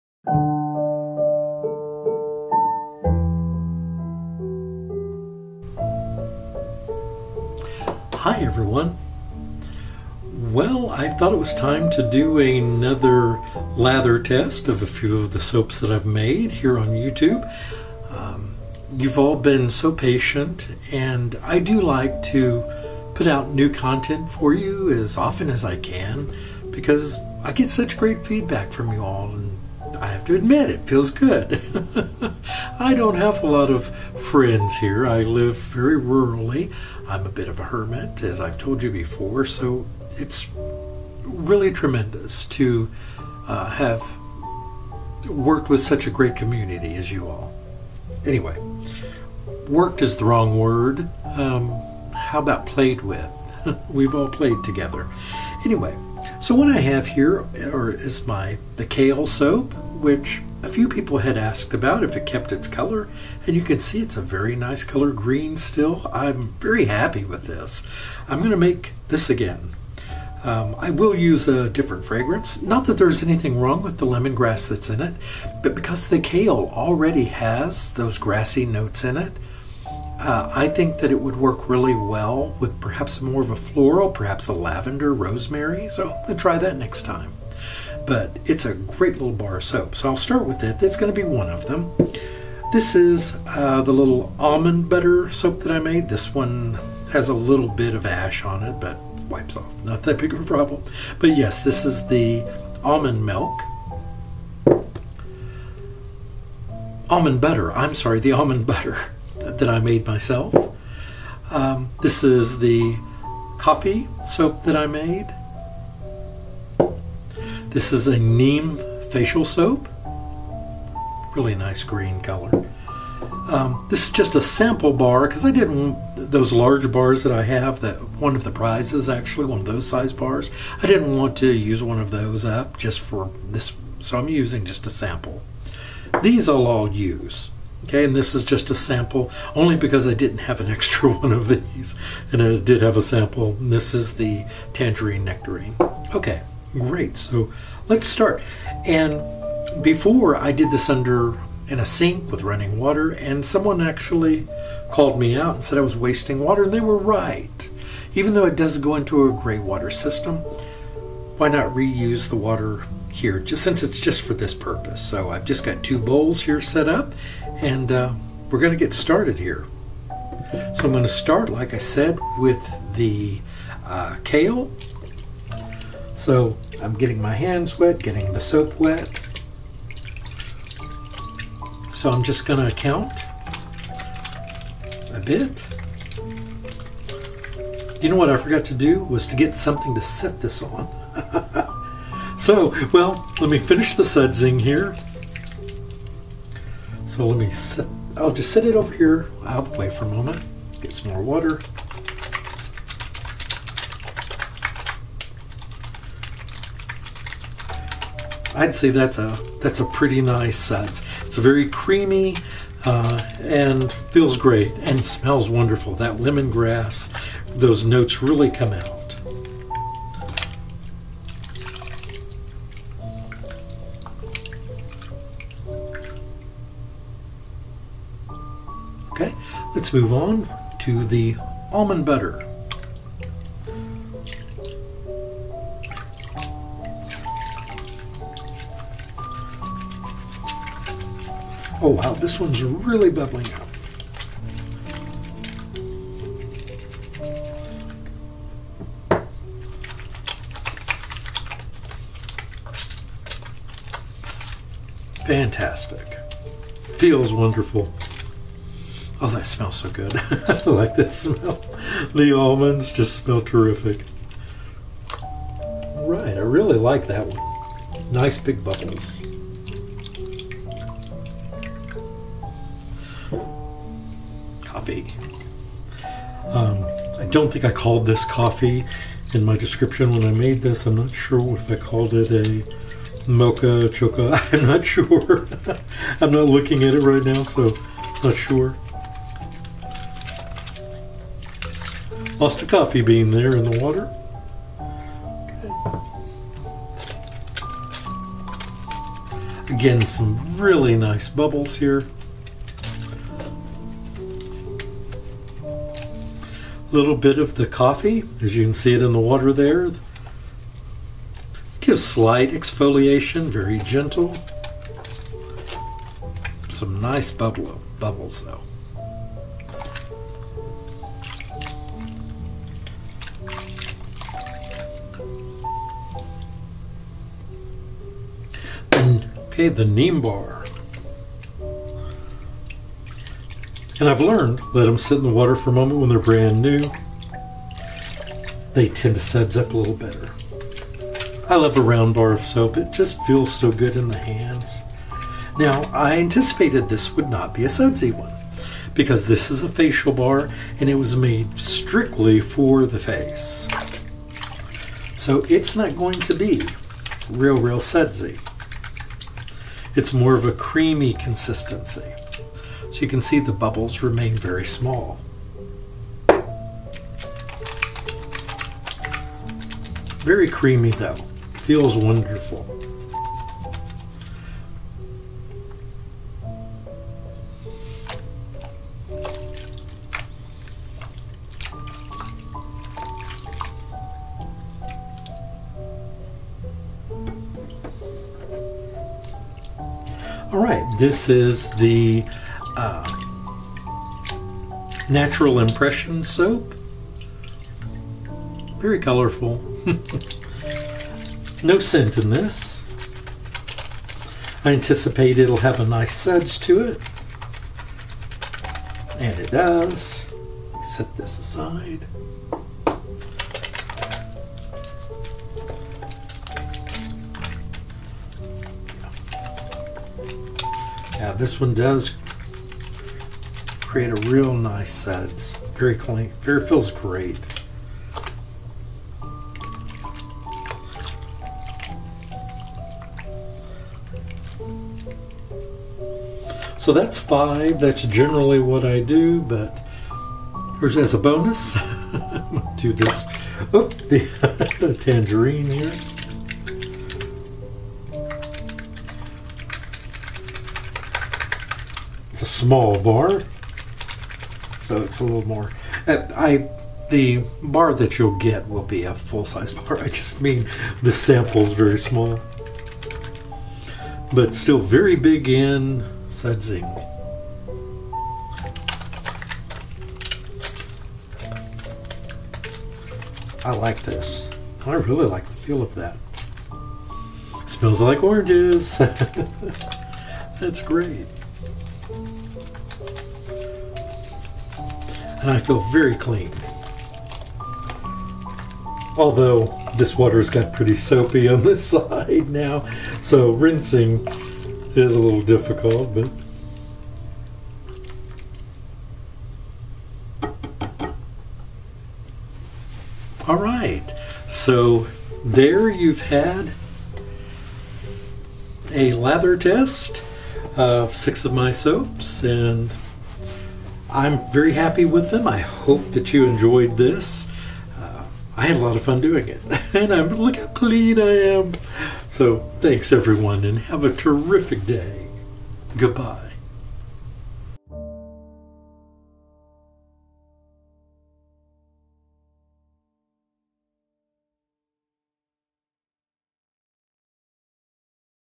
Soapy Oaks Farm LatherTest #2 - Handmade soaps (ASMR)
Lather/Suds test of 6 soaps. Watch me splash around and play in the water, and no one around to tell me to stop, isn't adulthood great?   Okay SECRET WORD #3 was in the video, I said next time I make the Kale soap that I would fragrance it with Lavender or ? Put the name of the other fragrance that I mentioned in a comment to be entered in the drawing.  This is the final word and I will draw a winner on Friday July 20th 2018.

Mold I use DD Life Flexible
Kaolin Clay
Tomato Powder
Spinach Powder
Squash/Pumpkin Powder
Lye (Sodium Hydroxide)
Cucumber Seed Essential Oil
Shea Butter
Coconut Oil